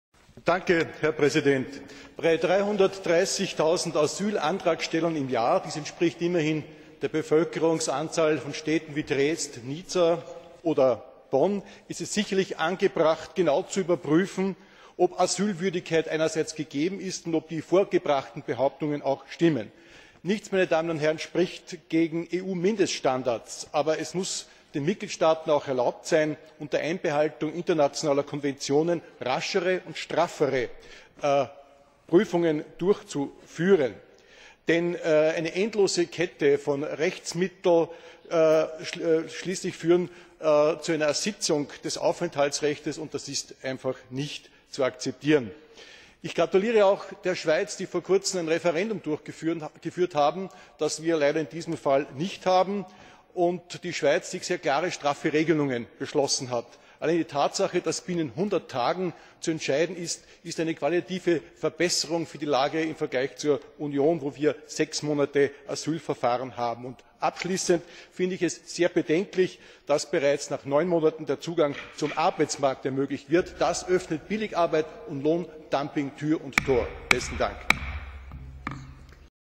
Franz OBERMAYR - Establishment of 'Eurodac' for the comparison of fingerprints... - 11-06-2013
Franz OBERMAYR - Establishment of 'Eurodac' for the comparison of fingerprints - Granting and withdrawing international protection (recast) - Laying down standards for the reception of applicants for international protection (recast) - Application for international protection lodged in a Member State by a third-country national or a stateless person (recast version) - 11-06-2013